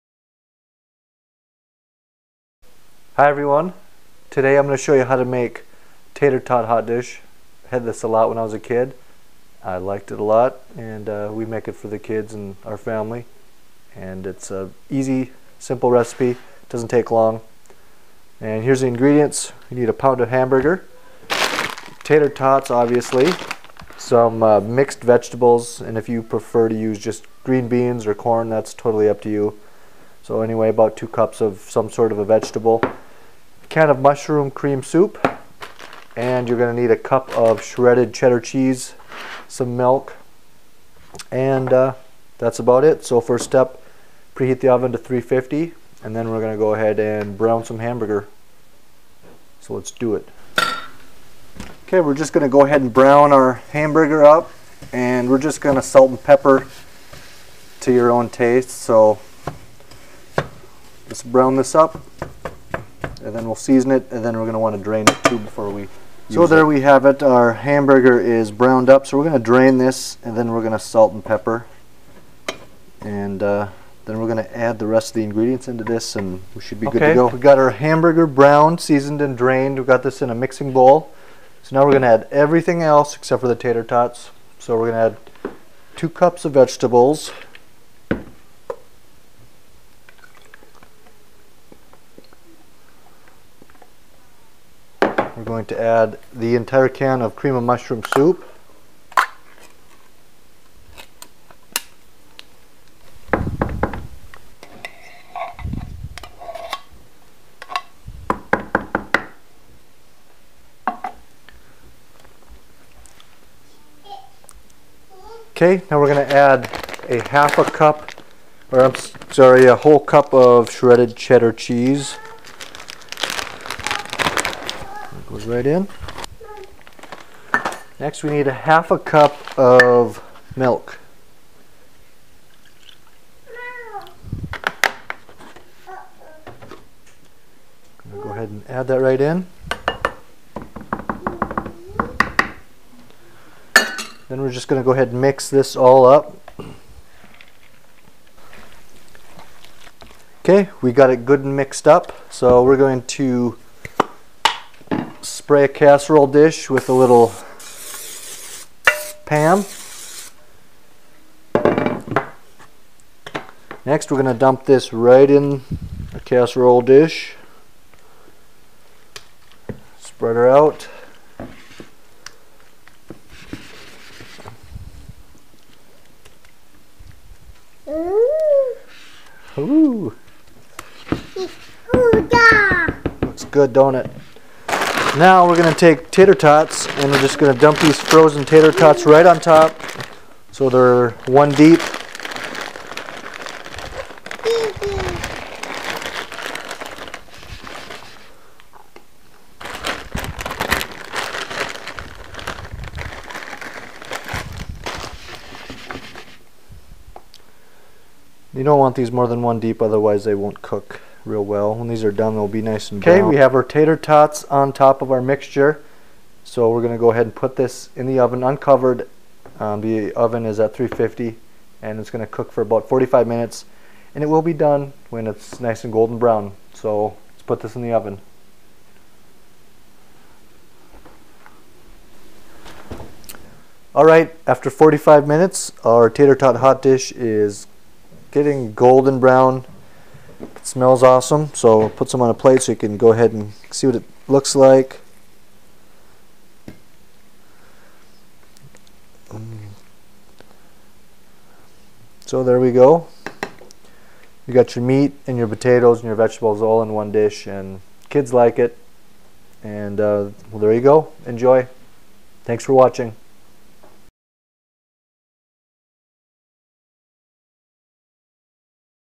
Tater Tot Hot Dish Recipe
Tater Tot Hot Dish Video Recipe for your viewing pleasure.  This Tater Tot Recipe with Cheese and Vegetables is a big hit in our house.  I hope you enjoy it.  Full recipe at: